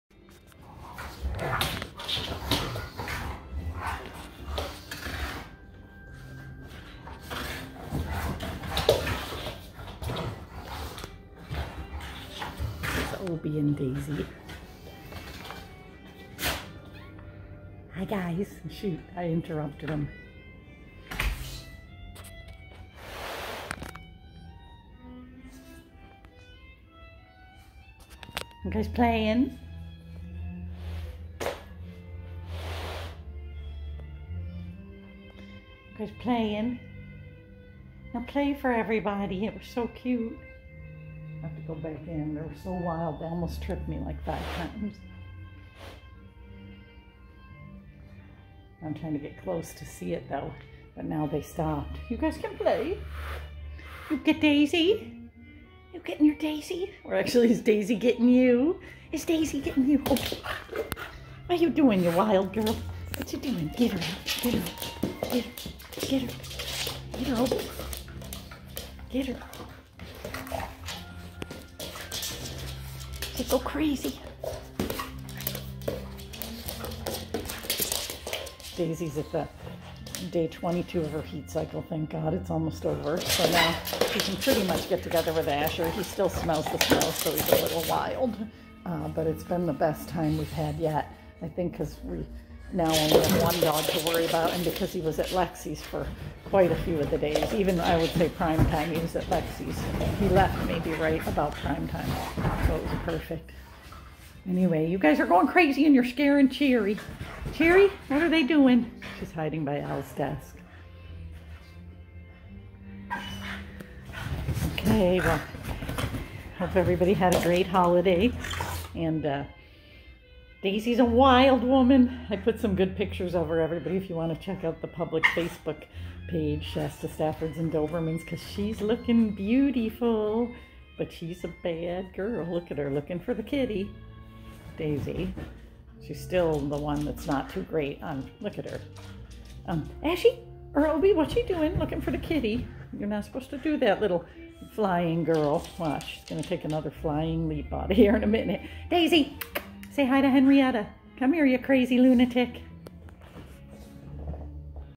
AKC STAFFORDSHIRE BULL TERRIER puppy and Doberman playing
9 mths old Daisy & 6 year old Obi-besties @shastastaffordsanddobermans